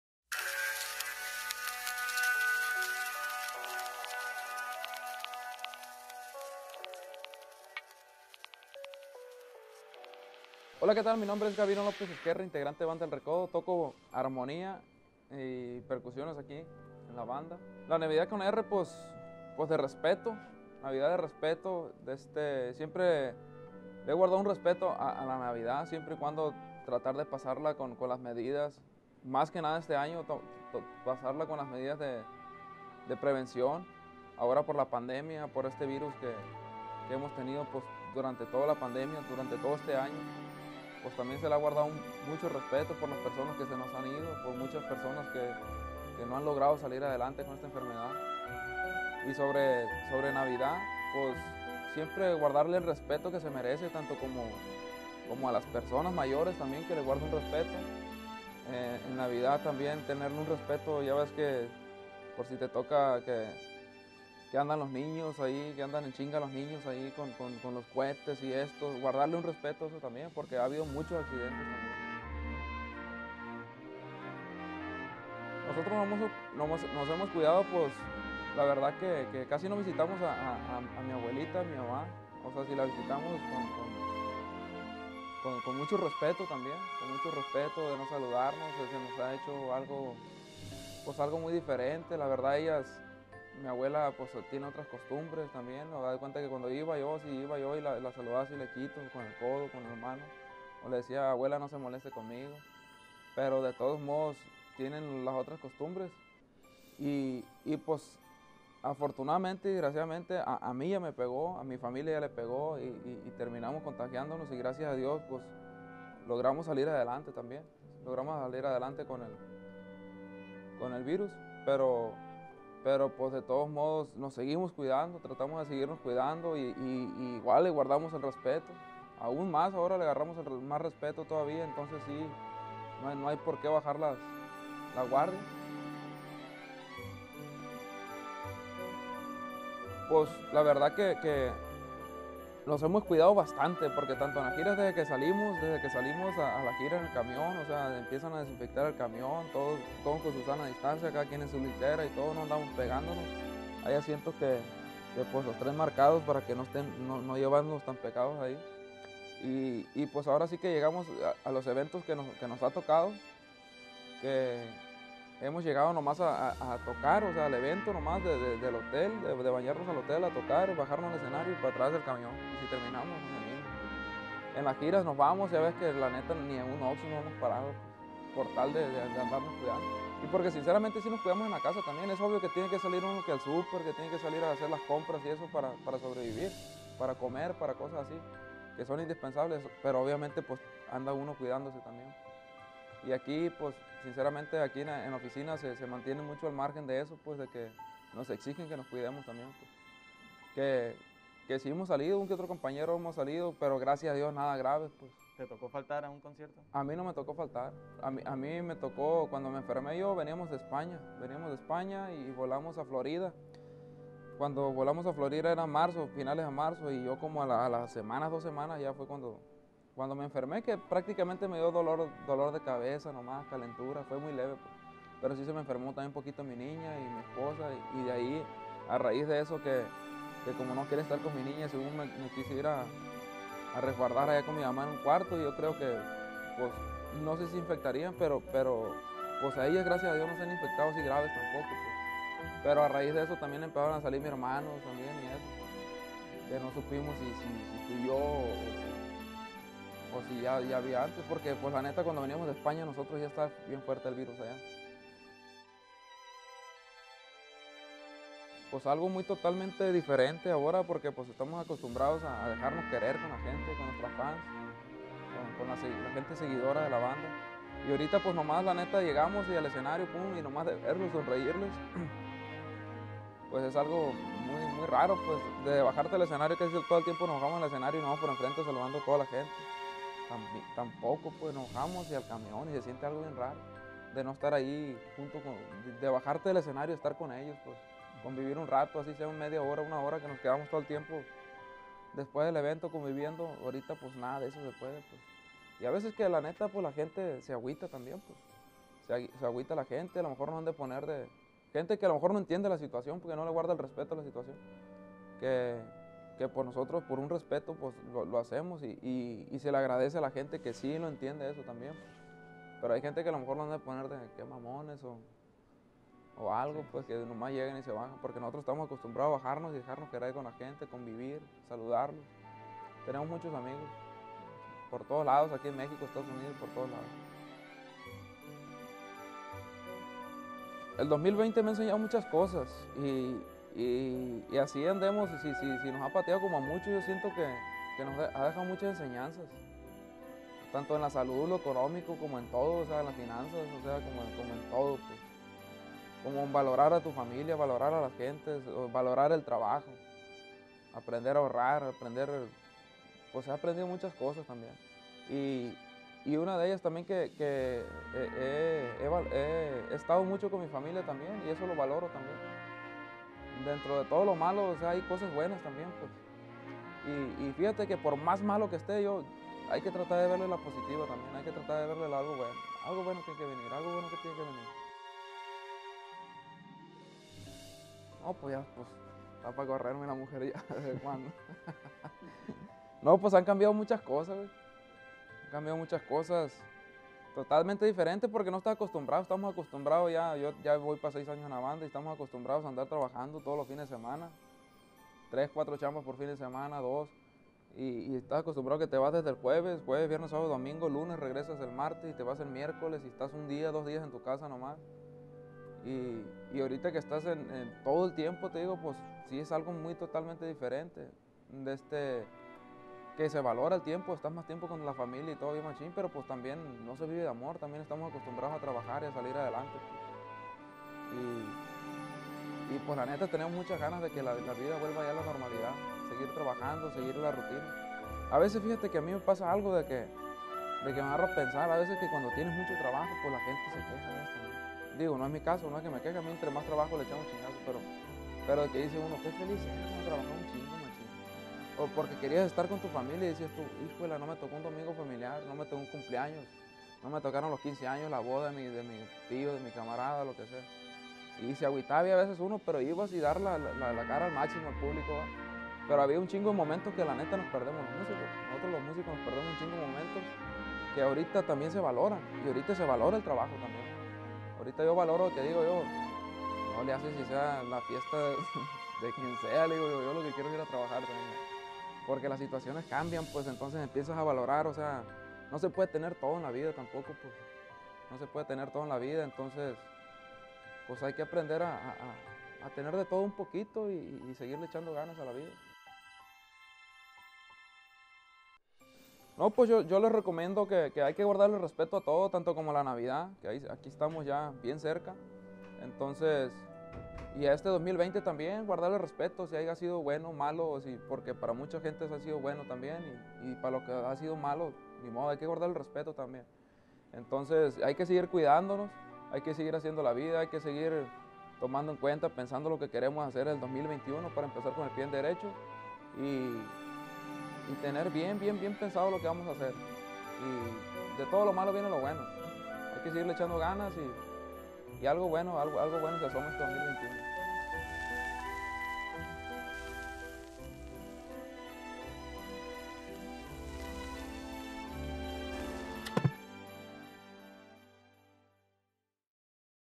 🔴NAVIDAD con R: GABINO LÓPEZ"🎄
¡Gabino nos cuenta como será su NAVIDAD con R... y nos comparte una reflexión de lo que el 2020 ha dejado en su vida!

COMENTANOS ABAJO EN LA CAJA DE COMENTARIOS